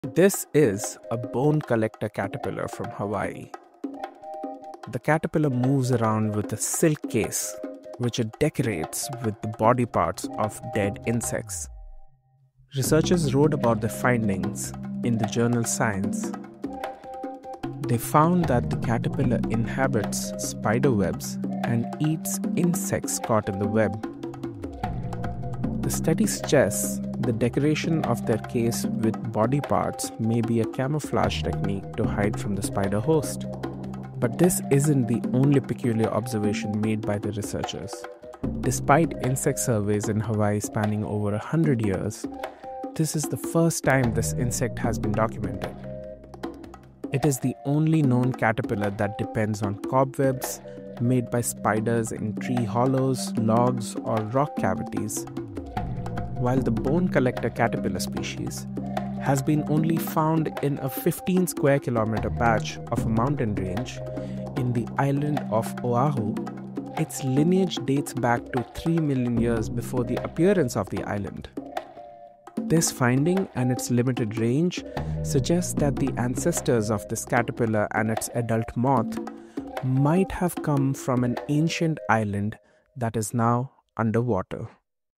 Scientists just made some unexpected discoveries about this 'bone collector' caterpillar from Hawaii
Read more about this rare species in Science journal.